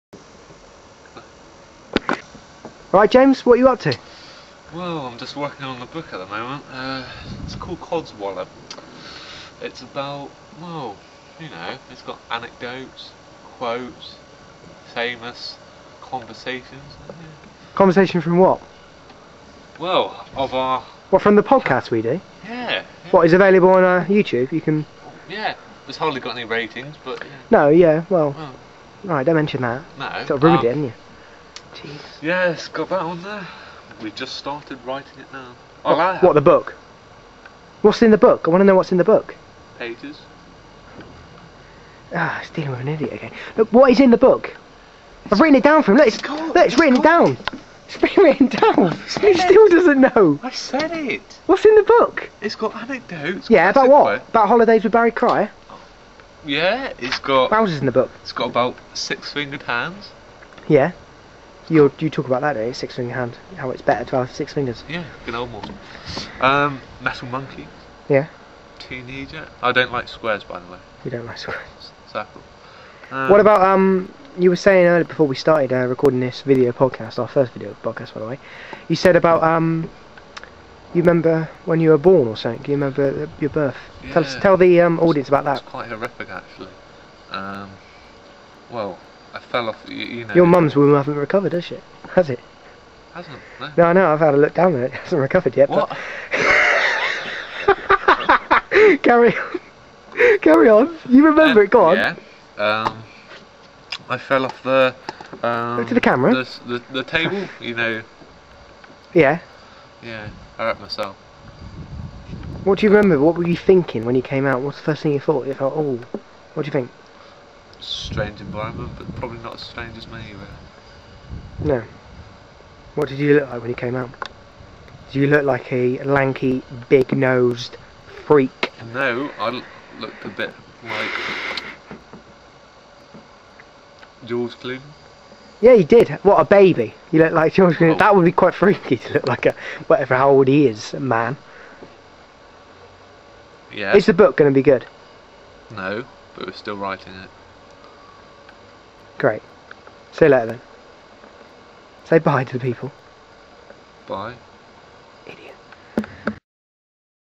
The James Cotter, James Coard Show First Video Podcast - Codswallop
James Cotter & James Coard have recorded there first video podcast where James Coard attempts to plug Cotter & Coard's new book 'Codswallop' but fails even though it was all written down for him. James Coard remembers his birth and talks about his freakish looks and Cotter & Coard discuss James Coard's mum womb.

This video podcast was recorded on Tuesday 23rd September 2008. This was there 1st video podcast and 44th show of all time.